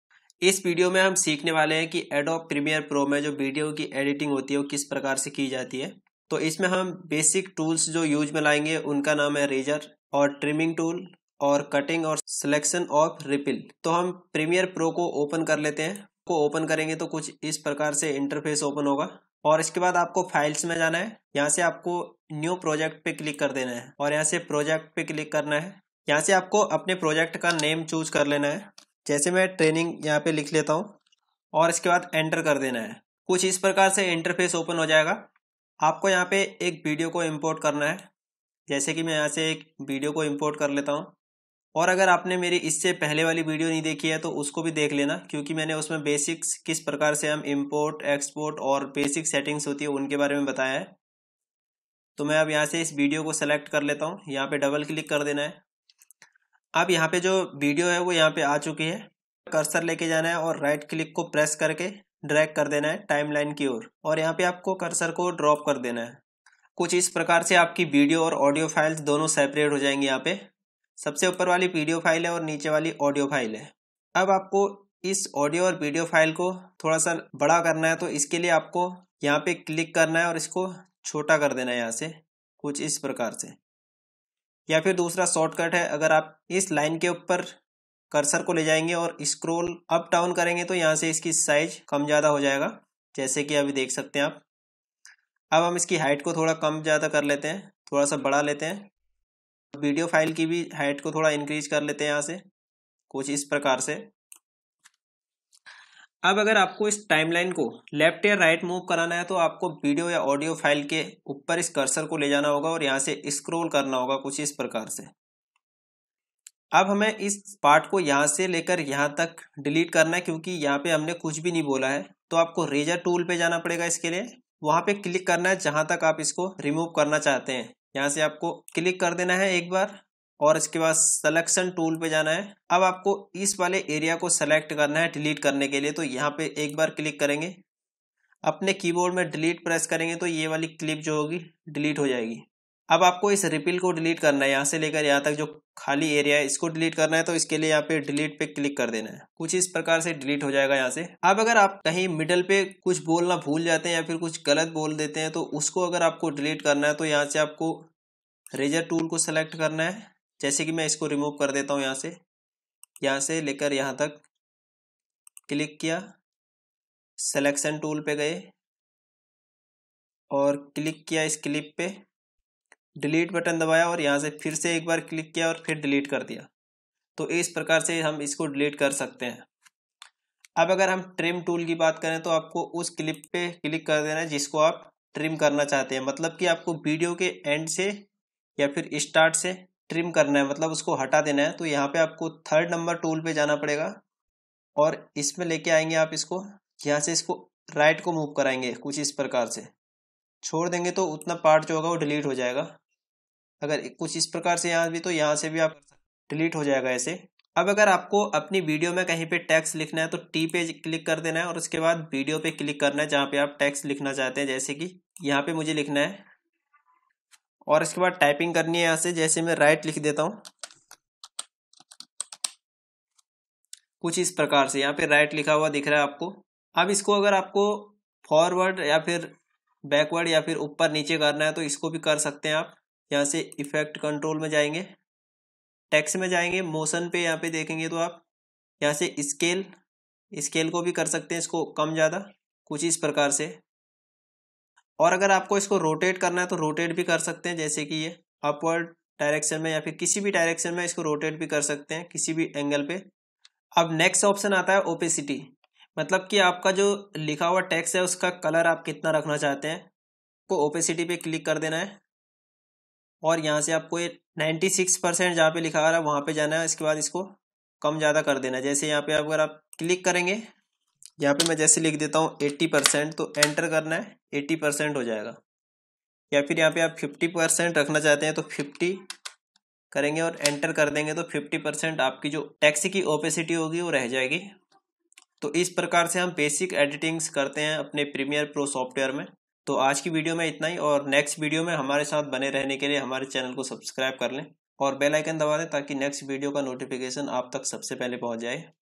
how to edit video in premier pro cc ??How to use cut trim and text tools?? basic video editing...
Basic video editing in premier pro software...
how to use trim cut text ripple tools in premier pro...